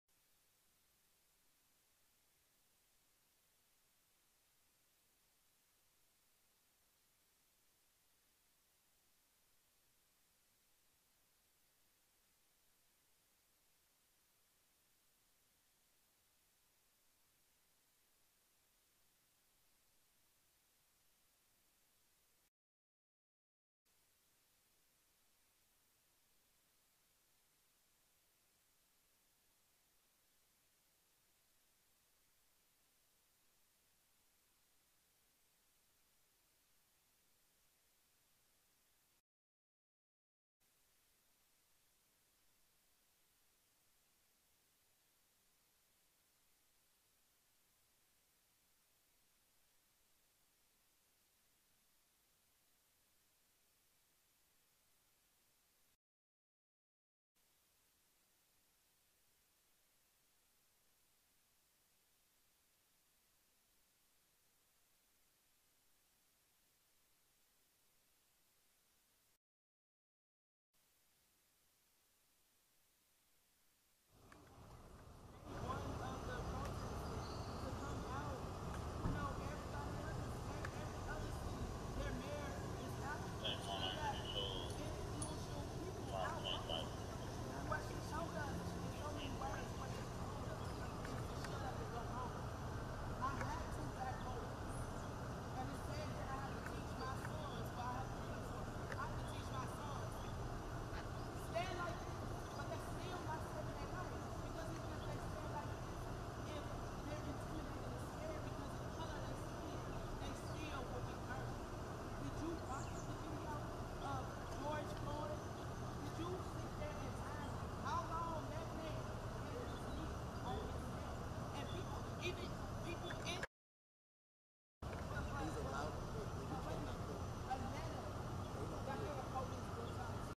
Protesters showed up outside Atlanta mayor's house
A group of about 10 peaceful protesters showed up outside the home of Atlanta Mayor Keisha Lance Bottoms early Saturday morning. They departed without incident.